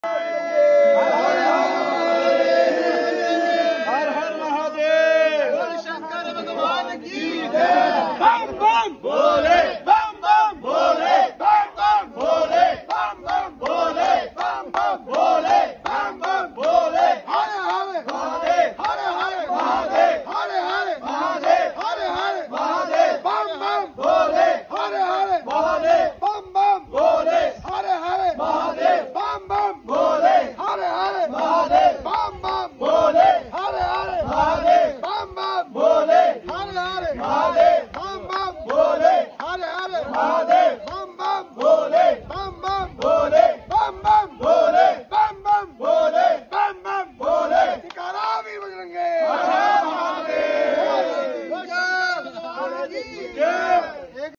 मणिमहेश भरमौर हड़सर
पैदल यात्रियों के खोला गया मणिमहेश भरमौर से हड़सर वाला रास्ता ।
Manimahesh Yatra 2022
Team:- @BholebabakifoujManimaheshGroup  Amritsar se
Admin:- @rajatgiftcardcenter
Sewak:- Sahib saloon ,Rajatgiftcardcenter,Raju T.v , Sumit Prabbkar 🙏🏻🙏🏻🙏🏻🙏🏻🙏🏻🙏🏻🙏🏻🙏🏻